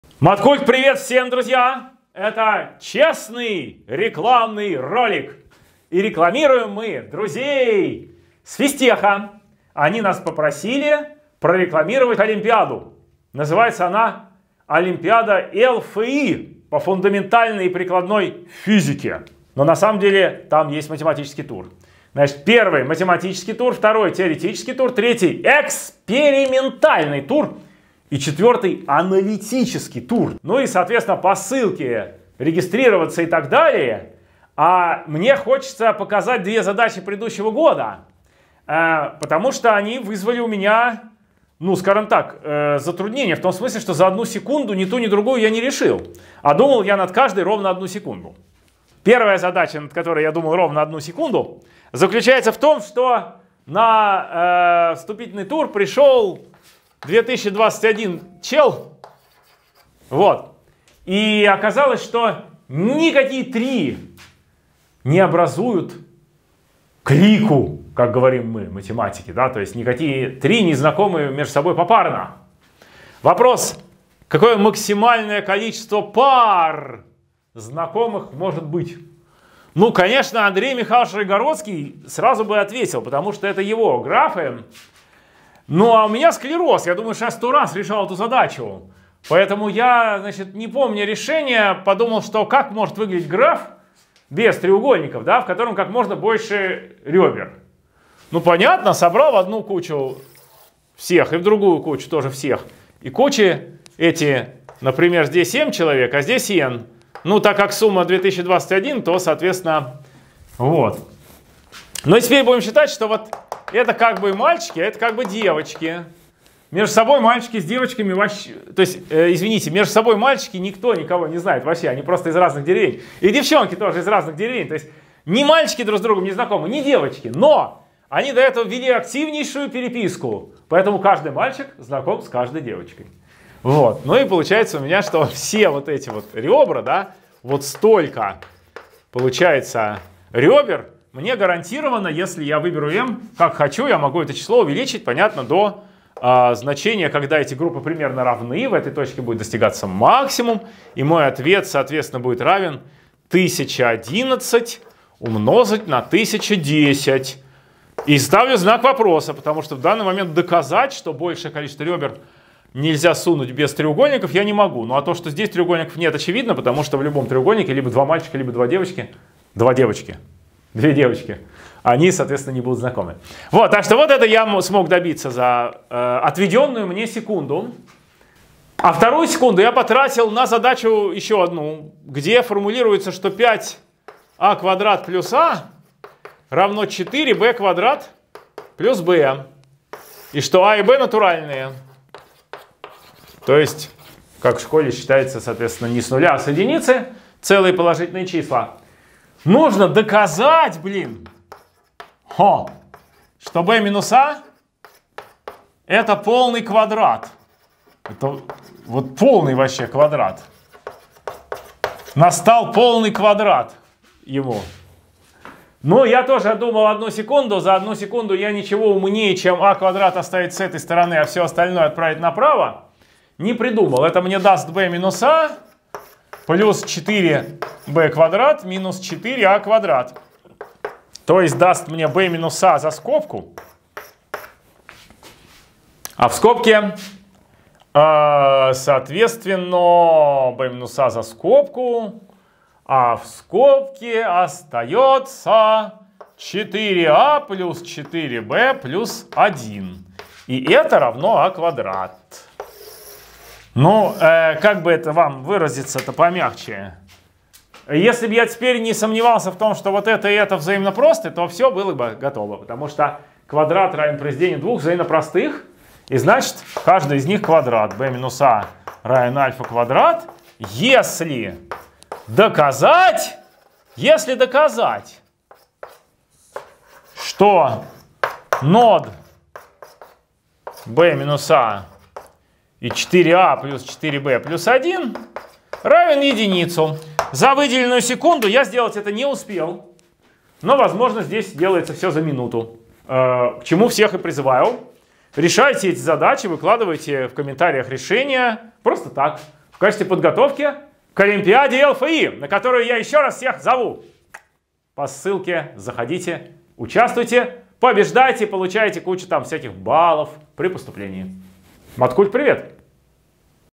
ПРИГЛАШЕНИЕ НА ОЛИМПИАДУ ЛФИ
Олимпиада ЛФИ по фундаментальной и прикладной физике 2022

Олимпиада ЛФИ по фундаментальной и прикладной физике — это олимпиада для 9-11 классов, которая проводится физтех-школой ЛФИ Московского физико-технического института.

В онлайн-этапе этой олимпиады 4 тура, всего на решение выделяется месяц: каждую неделю открывается по одной новой теме для решения.

О турах:

1) Математический тур;
3 задачи олимпиадного уровня по математике, похожие на те, что решают в сборниках, но с небольшой особенностью.

2) Теоретический тур;
Классика всех олимпиад по физике — теоретическая задача на описание явления.

3) Экспериментальный тур;
Понять физику задачи, придумать эксперимент из того, что найдется дома и, наконец, получить реальные физические результаты своими руками.

4) Аналитический тур;
Уникальный для школьной физики, но основополагающий для научного исследования этап работы: обработка готовых данных и получение из них физических законов на основе теории.

Примеры задач к каждому туру можно найти на сайте олимпиады прошлого года по ссылке

О призах:
Все участники, показавшие хорошие результаты, приглашаются на заключительный этап, где будут бороться за баллы в ИД при поступлении в ЛФИ (
победителям олимпиады – 4 балла,
призёрам II степени – 3 балла,
призёрам III степени – 2 балла)

Авторы лучших решений каждого из этапов в отборочном и заключительном туре получат ценные призы.

Как участвовать:
1) Зарегистрироваться на сайте abitu.net.
3) Решать открывающиеся задачи в течение месяца в свободном режиме.